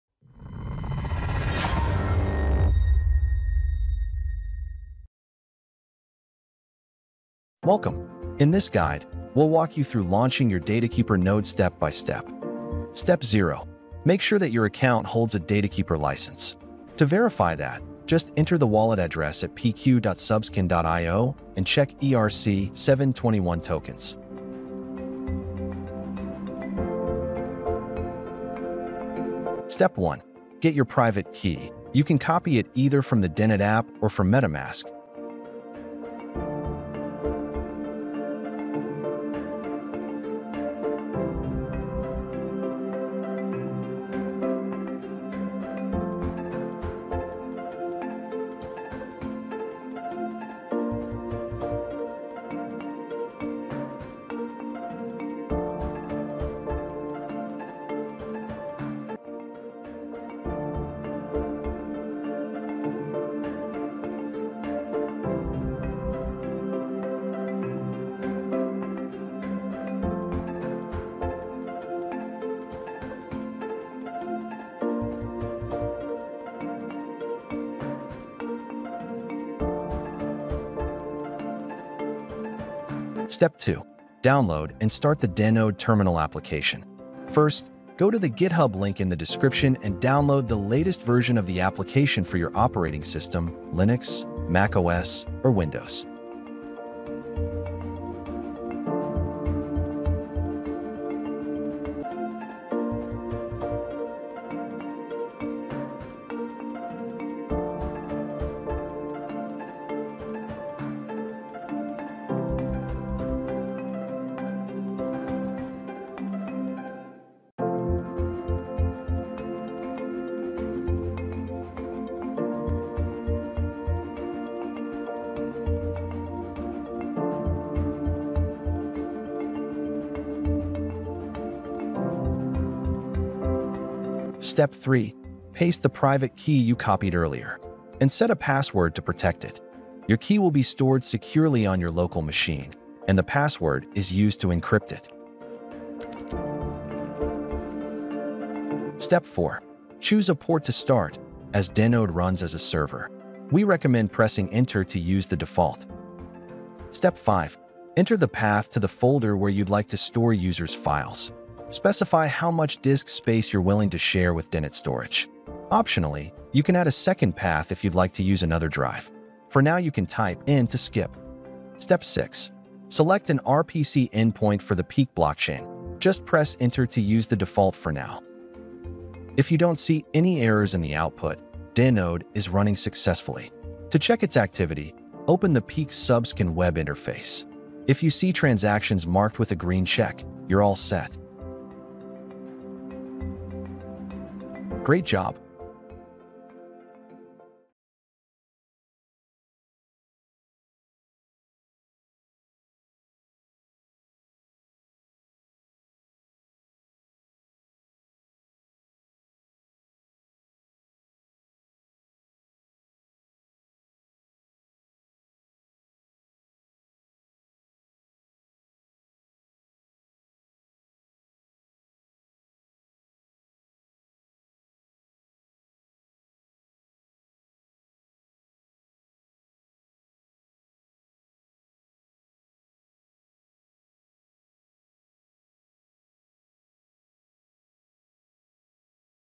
DeNet Datakeeper Windows Setup 🖥️⚙️ – Easy Step-by-Step Guide ✅
How to Launch Your Datakeeper Node | Easy Step-by-Step Tutorial

Get your Datakeeper Node up and running quickly with our clear, easy-to-follow video guide. The latest Datakeeper Node software supports Linux, macOS, and Windows — start contributing to the network today!

[CNMV: "Investing in crypto assets is not regulated, may not be suitable for retail investors, and you could lose your entire investment.”